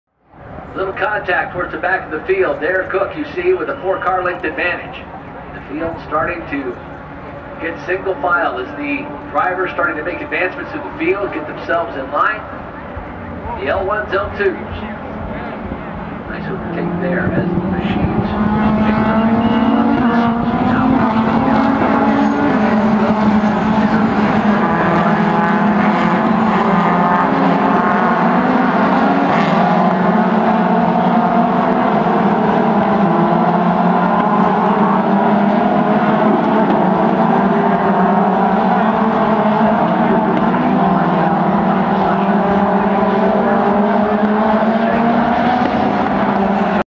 Indy Grand Prix of Louisiana - Start of Cooper Tires Prototype Lites Race
Moving up to our seats for the rest of the day, this video was shot during the opening lap of Prototype Lites event as the cars pass through turns 10-13.